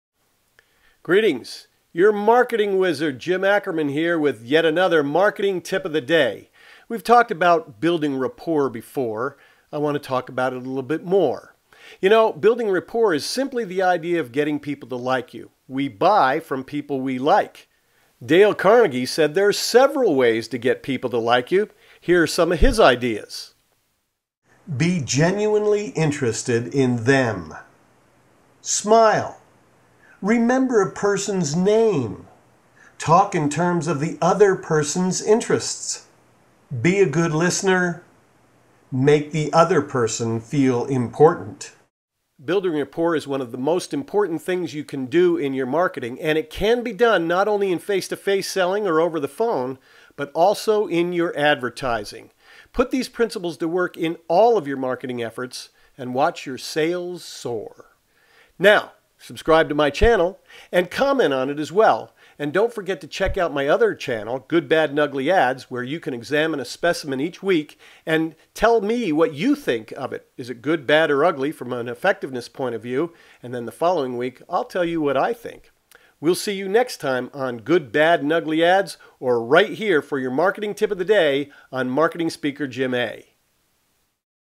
Marketing Tip o'the Day... More on Rapport
Marketing Speaker Jim Ackerman returns with yet another Tip o'the Day talking about establishing Rapport with your prospects. Face-to-face sales, telemarketing, advertising of any kind, you're better off talking to your prospects the way they talk. "We buy from people we like," Ackerman says, "And we like people who are like us..." Enjoy the tip!